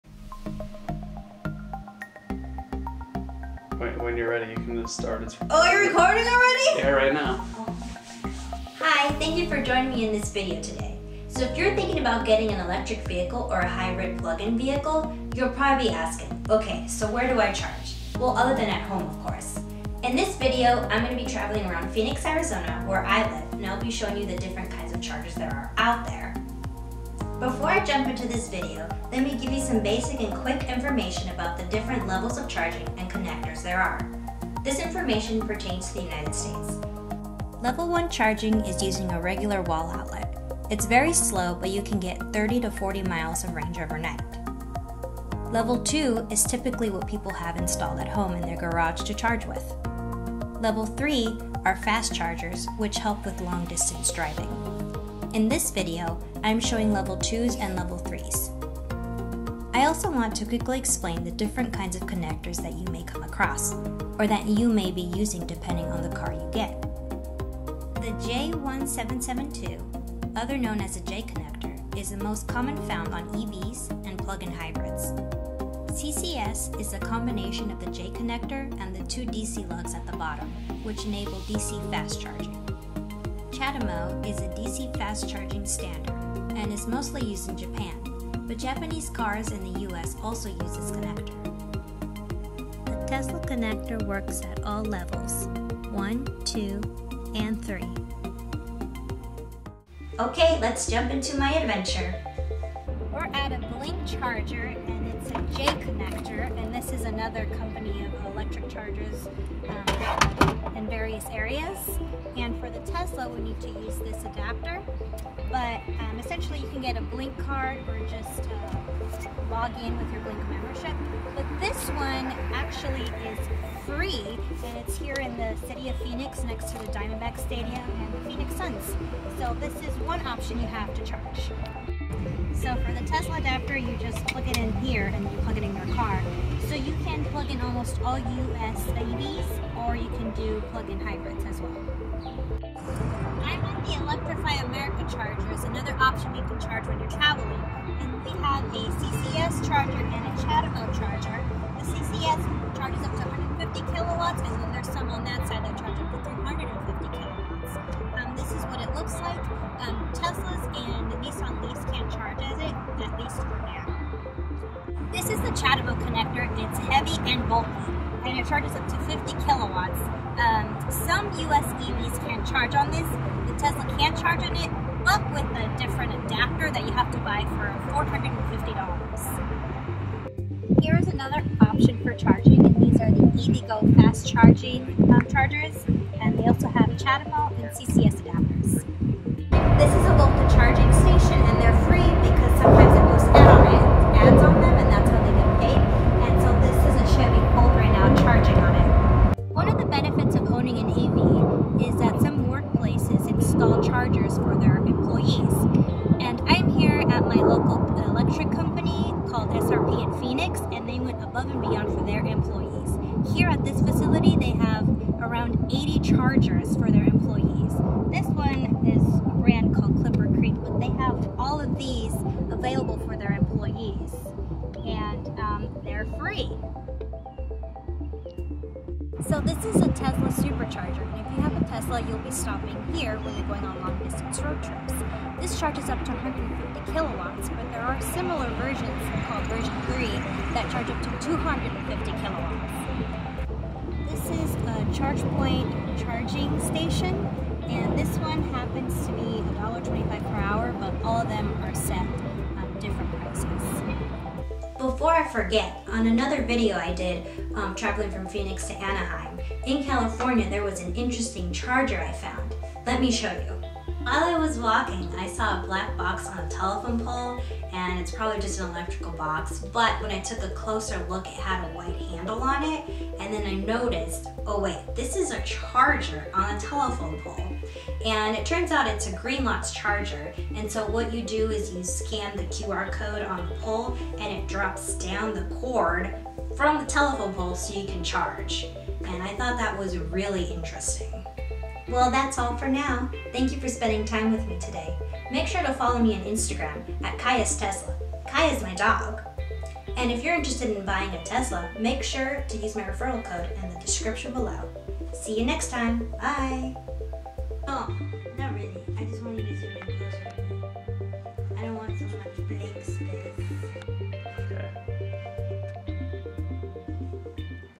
Where do I charge my Electric Vehicle?
I show different options to charge your electric vehicle when your traveling or away from home. Depending on the electric vehicle you own, you'll be able to use certain types of charging connectors. In this video I show you the majority of chargers that are available in Phoenix, AZ. I describe the connector types including CCS, CHAdeMO, J-Connector and the Tesla connector.

Liked Lorraine & Calla? Use her code! (M3 SR+ Owner)
Liked the editing? Use Alex's code! (M3 LR Owner)